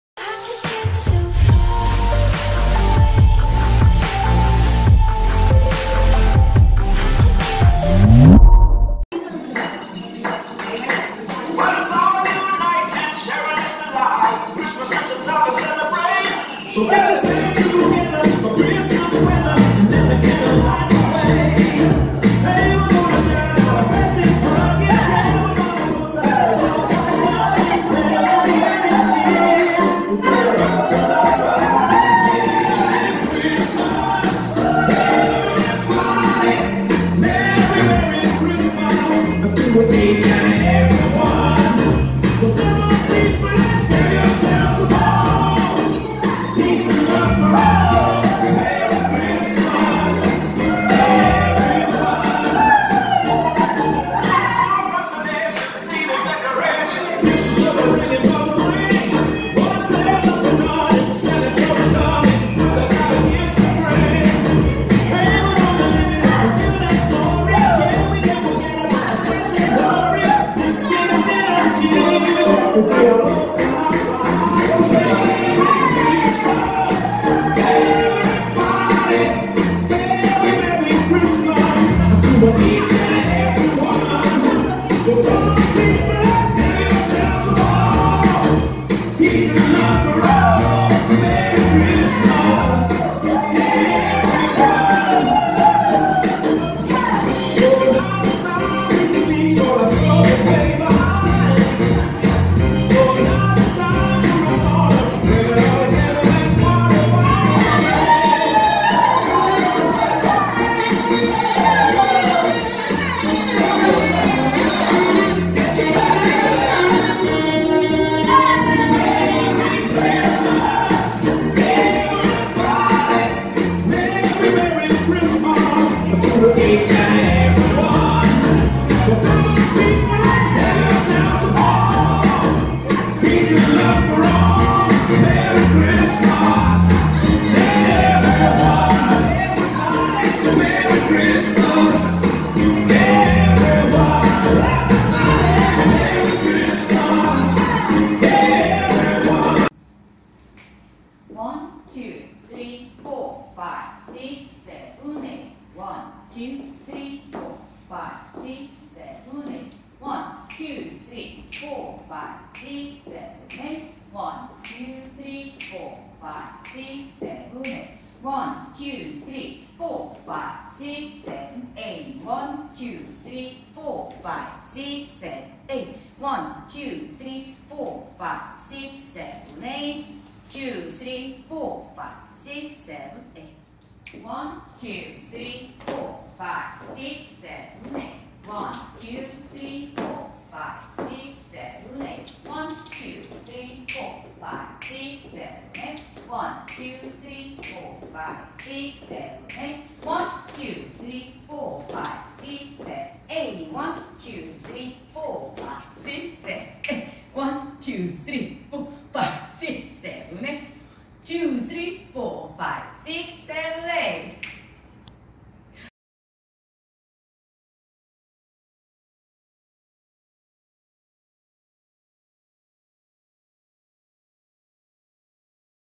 A Christmas Twist - line dance (demo & teach)
Count : 64       Wall : 2        Level : Beginner Contra

Choreographer : Junghye Yoon, Linedancequeen Korea (November 2017

Dance : Asan city South Korea - JMP Class (온양4동)